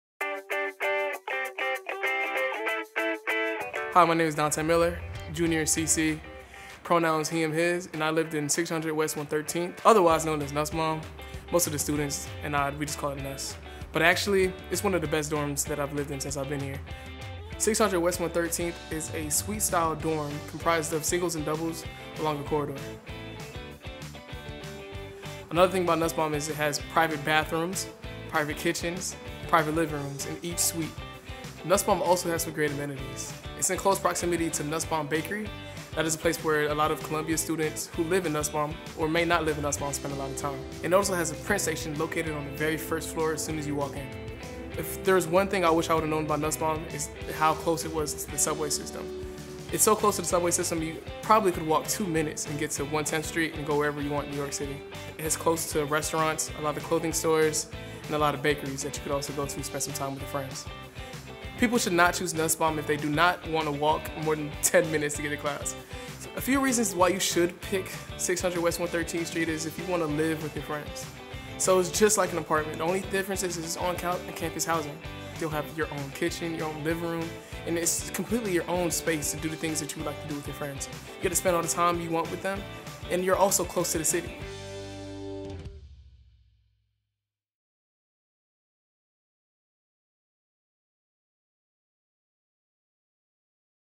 Room Selection Housing Tour: 600 West 113th Street
Take a virtual tour through 600 West 113th Street as former resident Dante describes the building's amenities and shares what his experience as a resident was like.